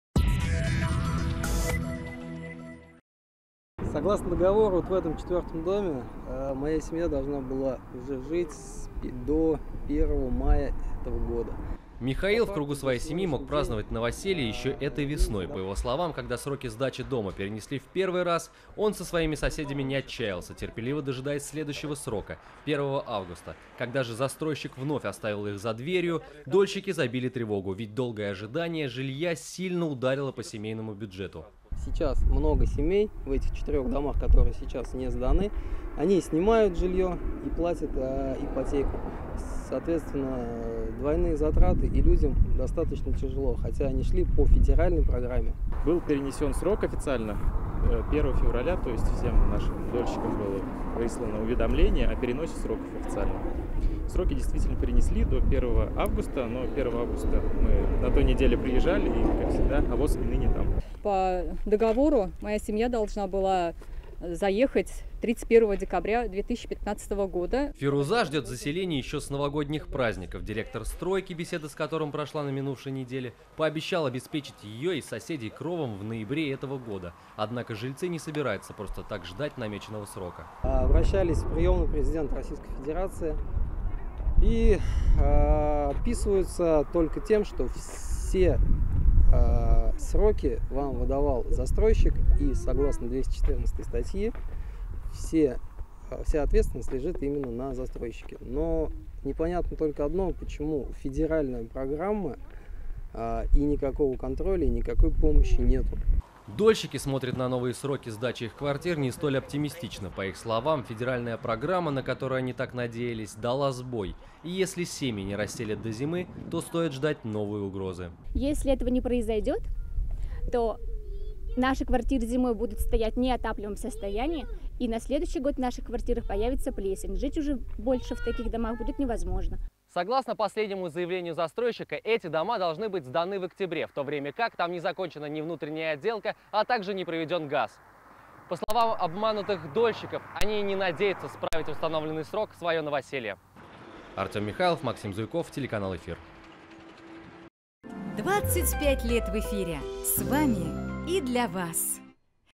Дольщики в Казани ждут заселения в дом микрорайона «Яшьлек» с мая
Доверились государству и пожалели. Ещё в прошлом году дольщики строящегося микрорайона «Яшьлек» в Казани вступили в федеральную программу «Жилье для российской семьи». Сдача домов в эксплуатацию была назначена на 1 мая.